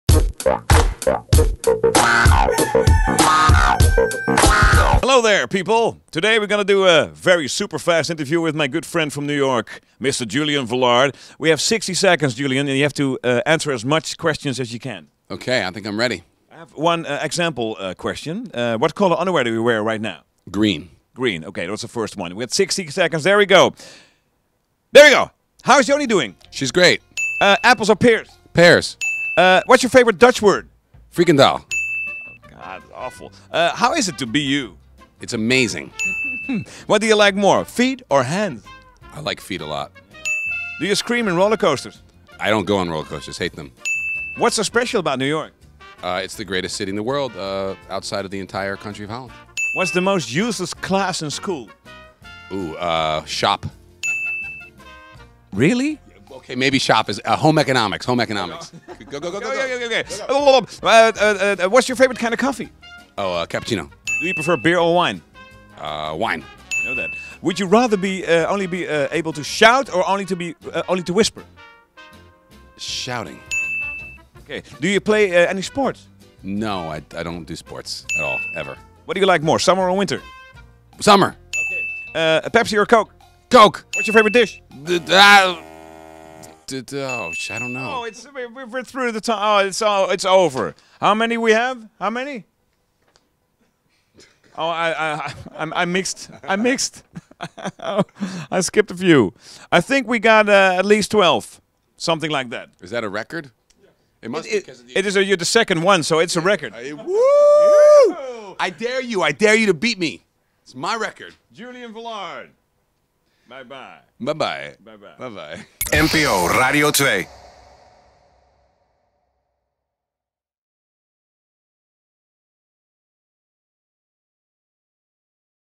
Supersnelle 60 seconden met Julian Velard
We hebben zestig seconden, een enorme rits aan vragen en een lopende camera voor een diepte-interview. Dit keer in de Supersnelle 60 seconden: Julian Velard.